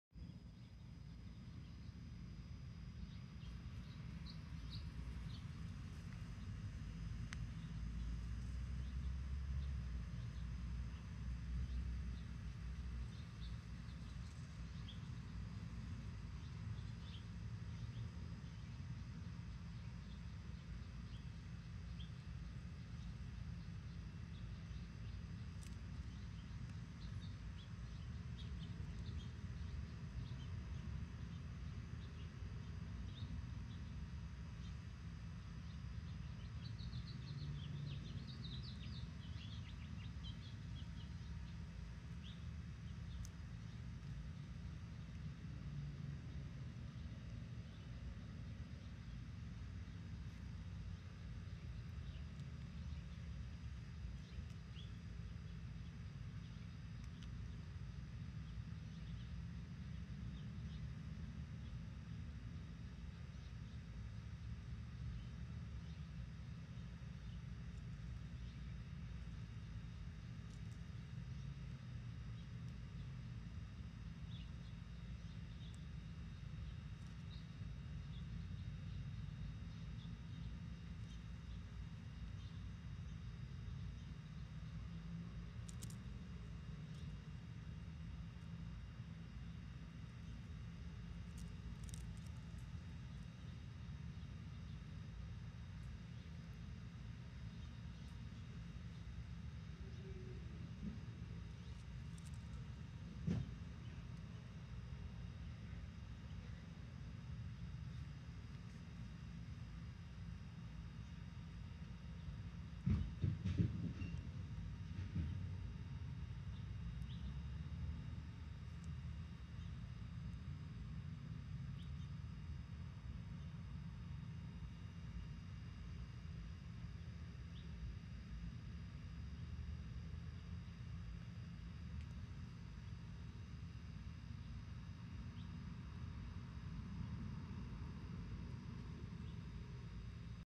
Turbo Dave chomping some clover
Dave the tiny tortoise chomps some clover